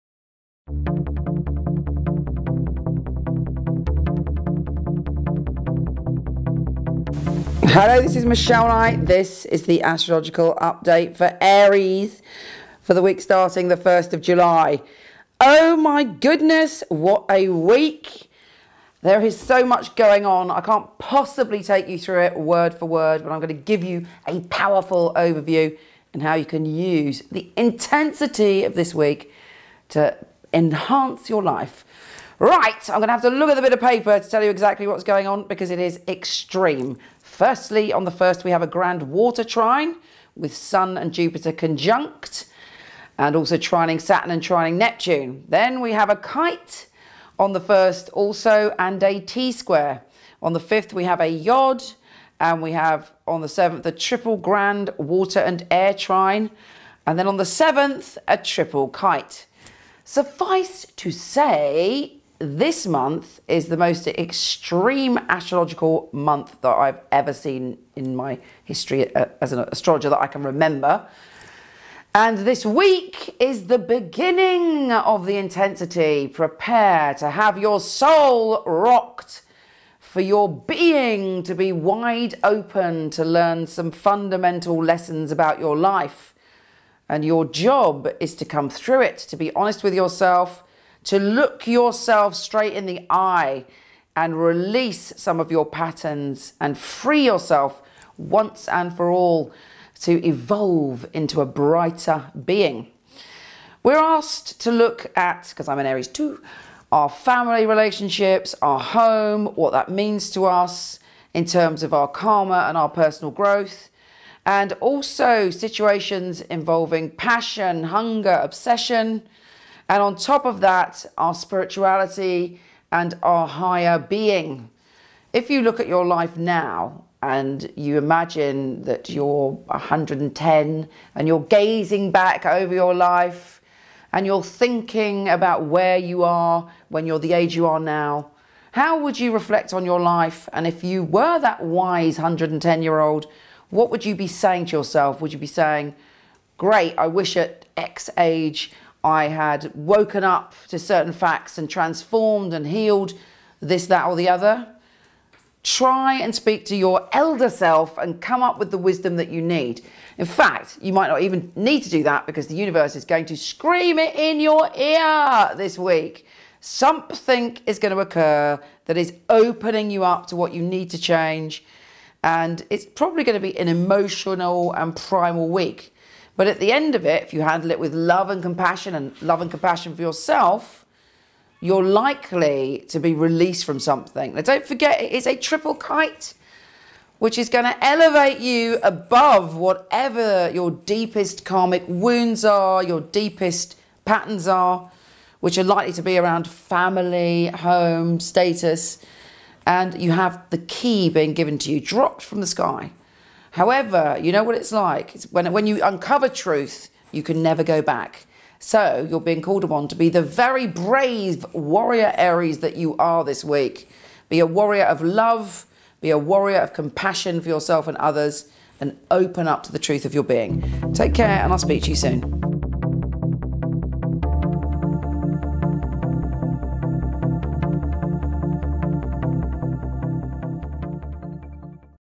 Aries Weekly Astrology 1st July 2013 with Michele Knight
Aries astrological overview for the week starting July 1st with Michele Knight.